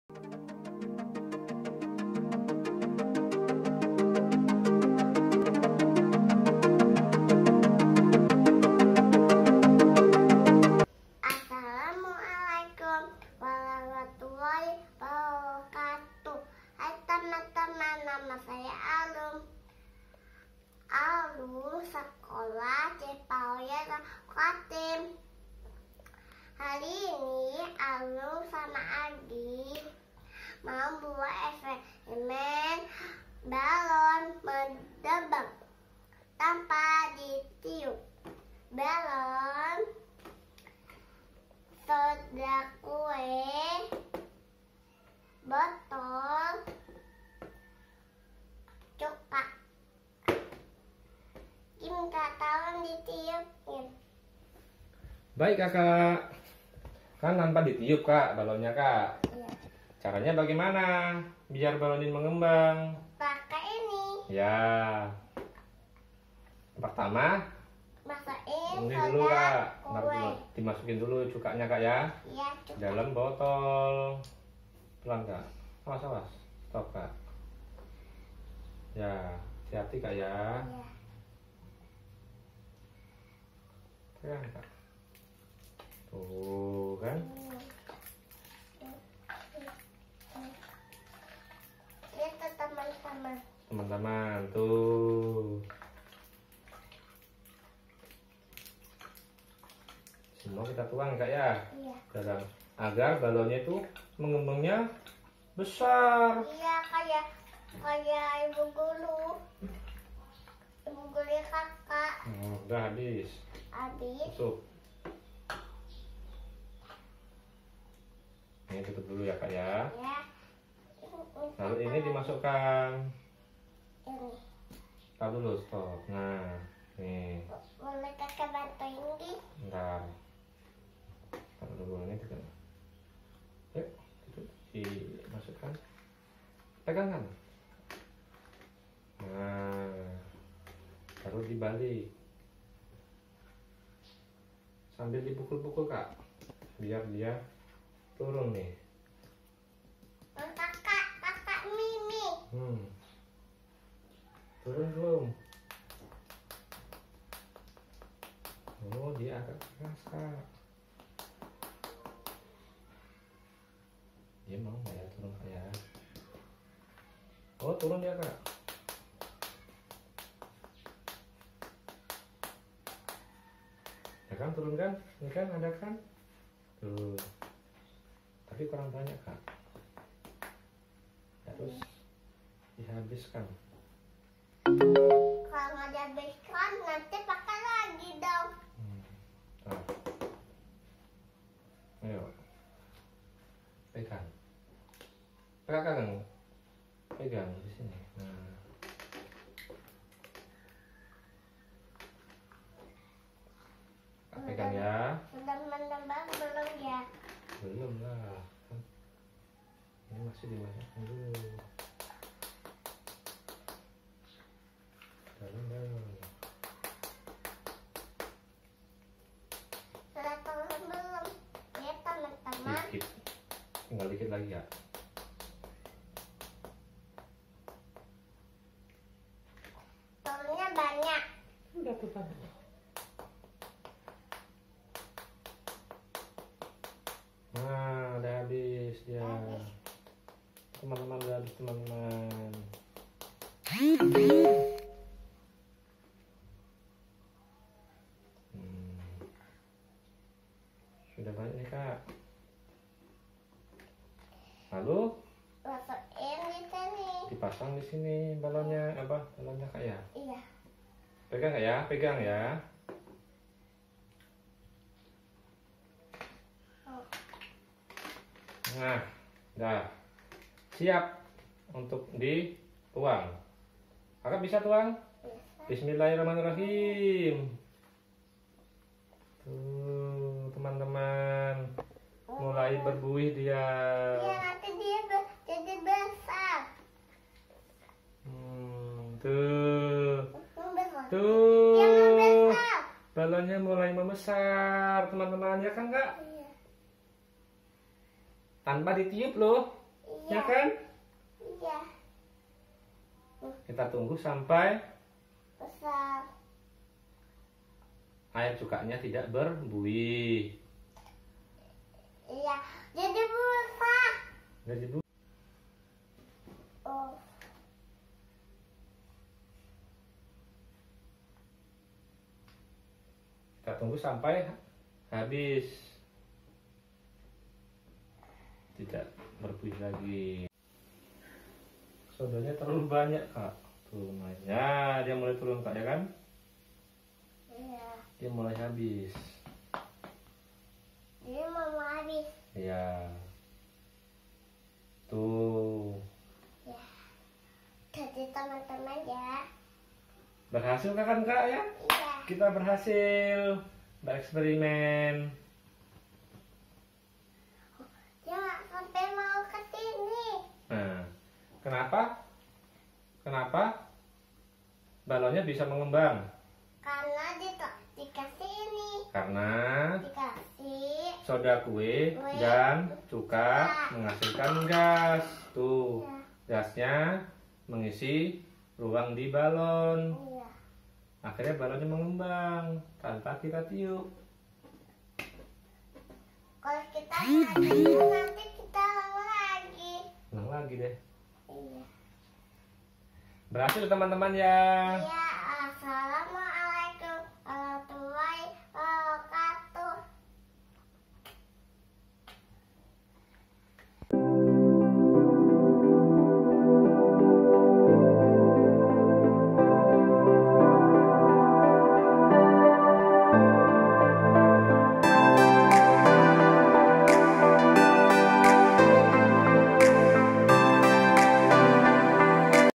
PERCOBAAN BALON MENGEMBANG TANPA DITIUP
Kali ini aurum dan agi melakukan percobaan balon mengembang tanpa ditiup. Salah satu materi pembelajaran PAUD TERPADU YPK Bontang. Alhamdulillah berhasil loh teman-teman.
Teman-teman semua juga bisa mencoba sendiri di rumah...asyik loh...

Terima kasih banyak teman-teman sudah menonton videonya aurum. Sampai berjumpa lagi di video yang lain...